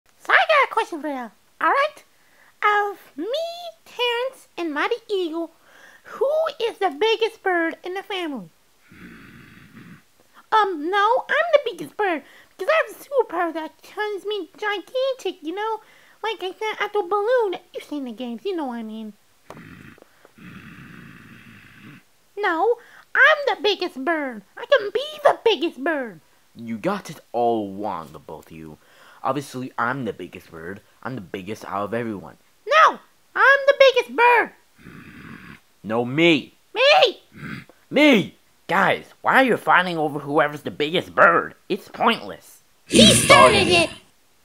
Who's The Biggest Bird?
3 birds on _______ Island try to prove that their the biggest bird.